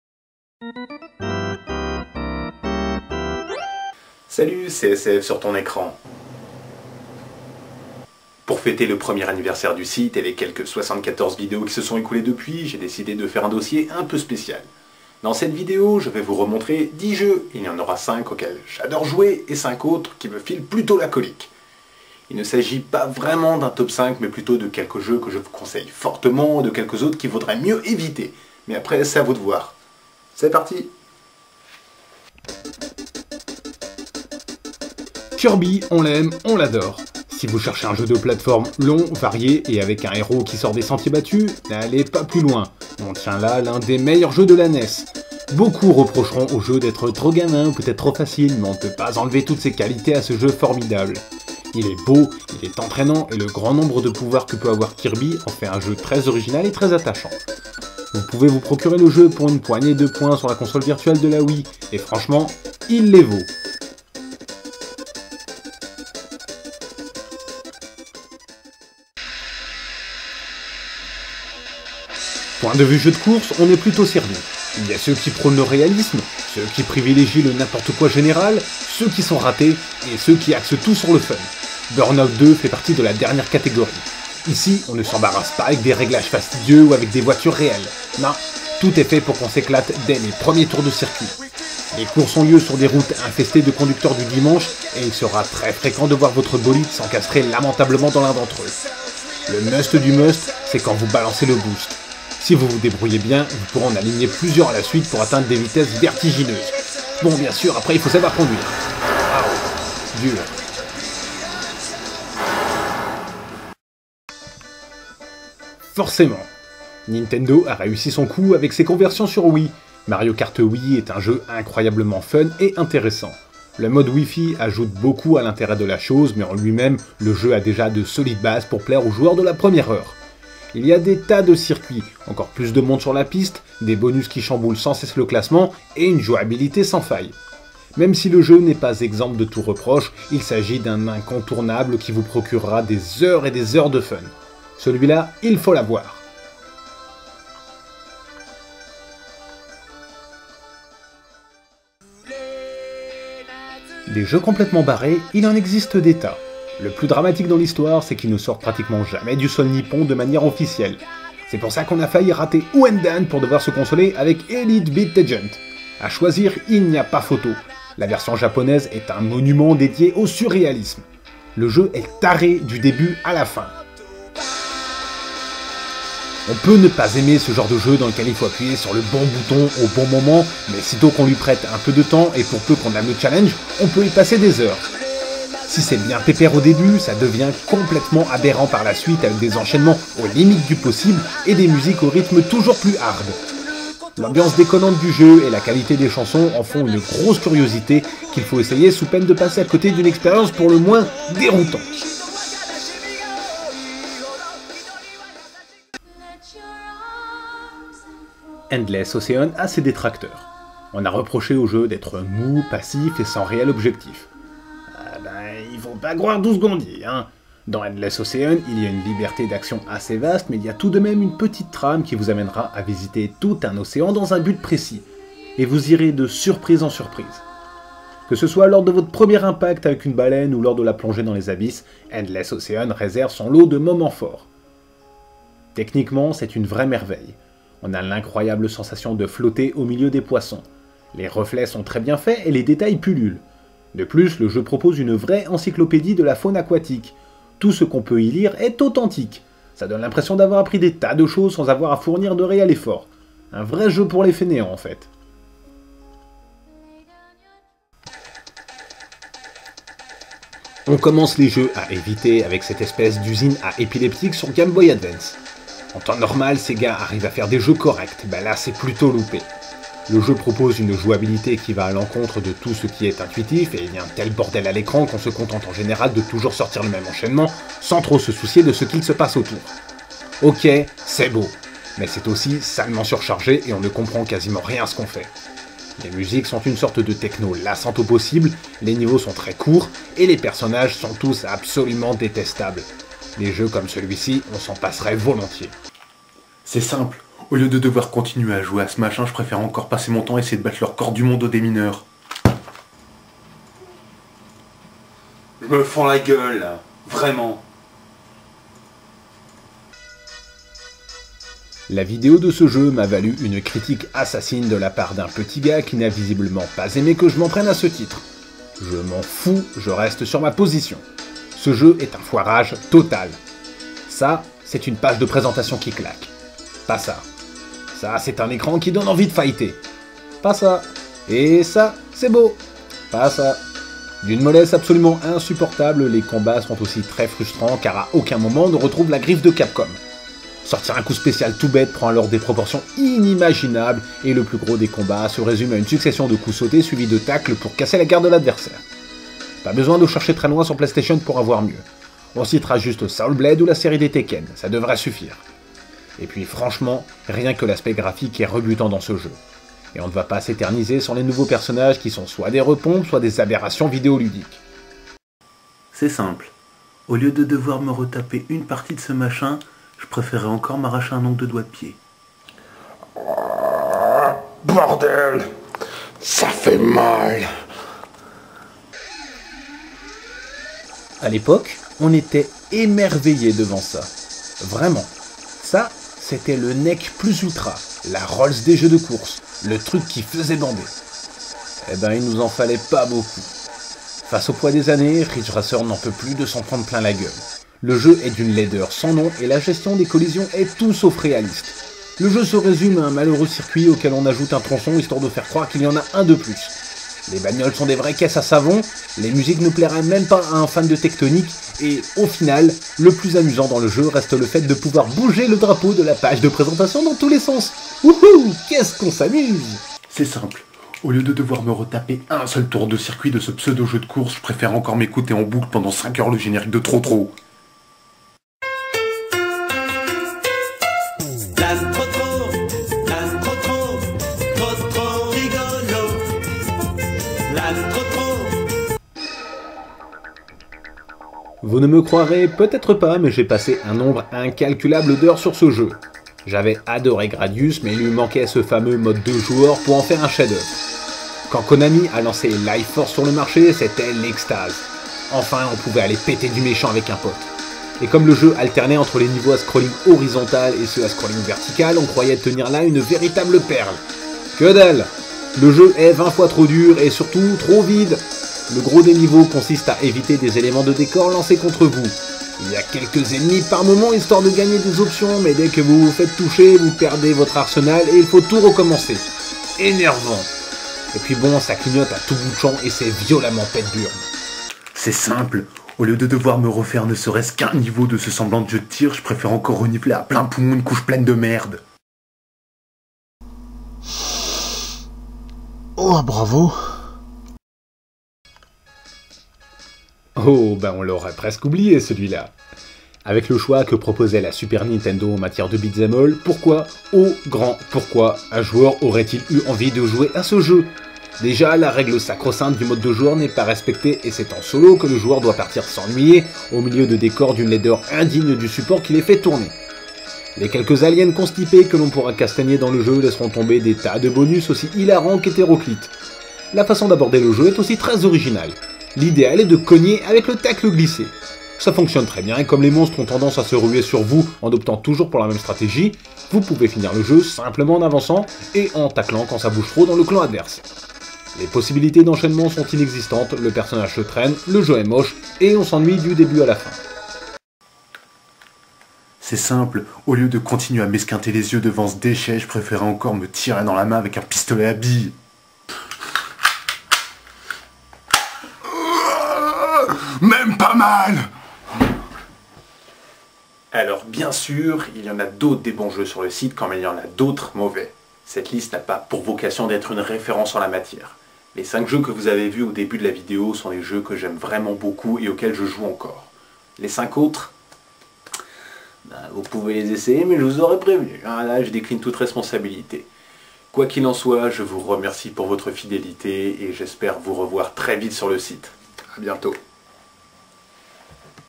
Dossier - Le premier anniversaire
Déjà un an que je publie mes vidéos sur le net ! Ce dossier spécial vous présente 10 jeux qui ont déjà eu droit à une vidéo sur le site. Il y en a 5 que j'adore et forcément 5 autres que je ne place pas très haut dans mon estime !